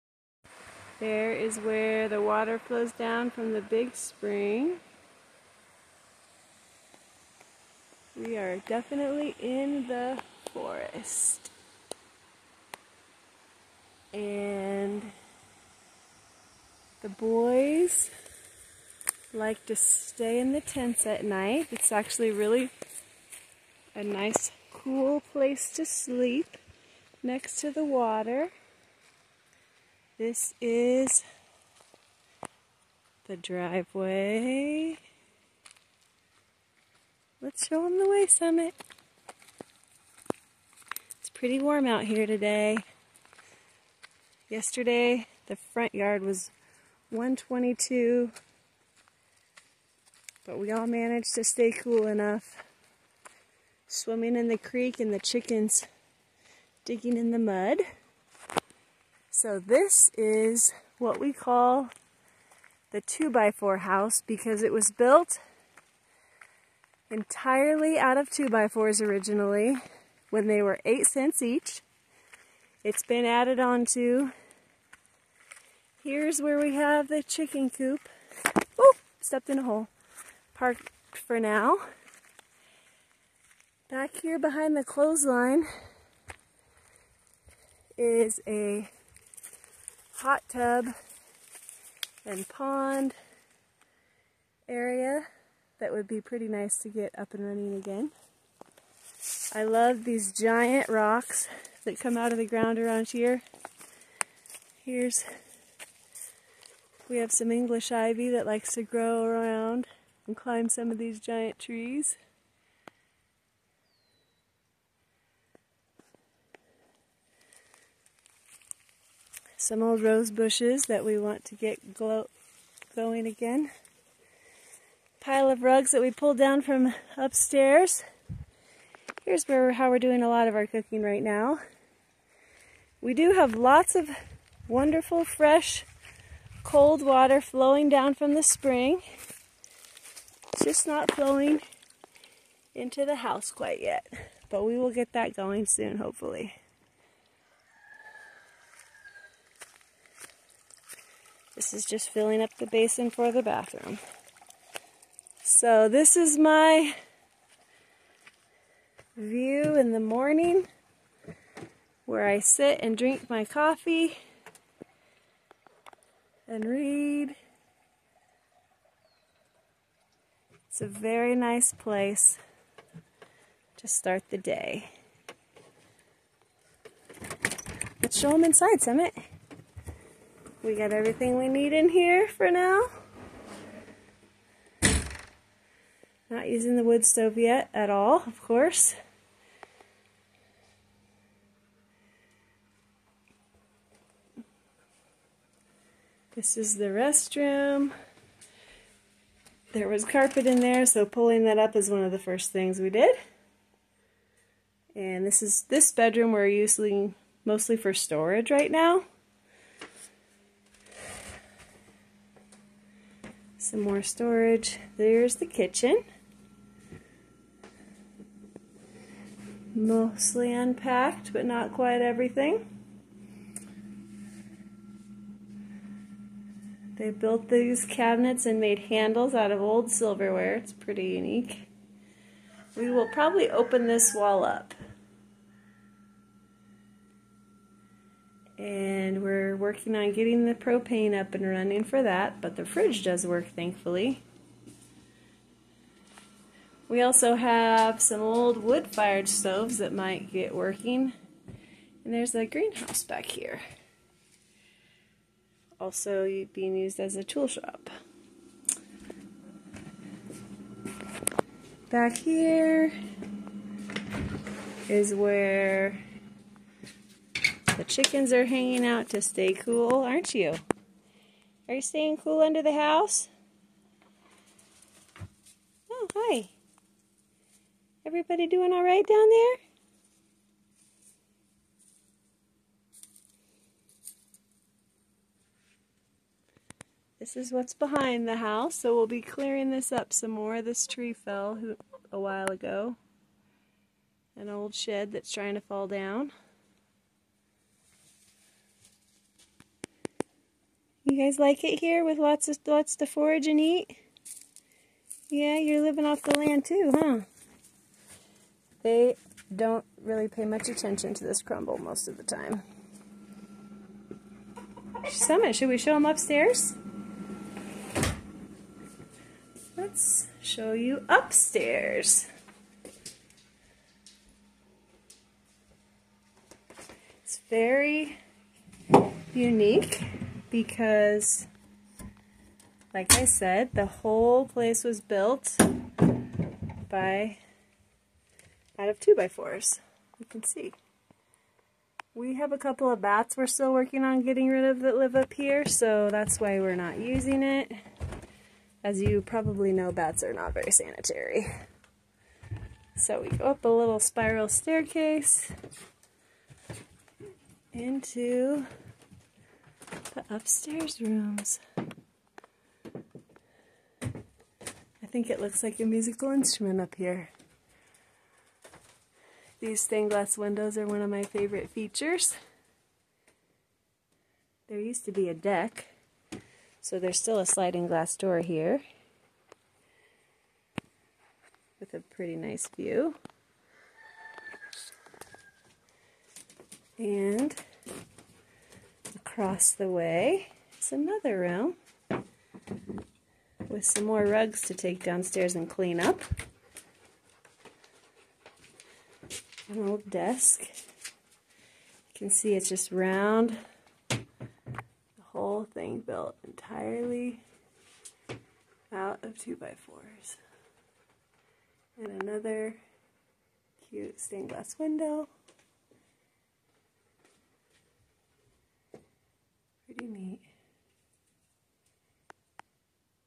2x4 cabin tour
Here is a video tour of the cabin we are camped out in for the time being.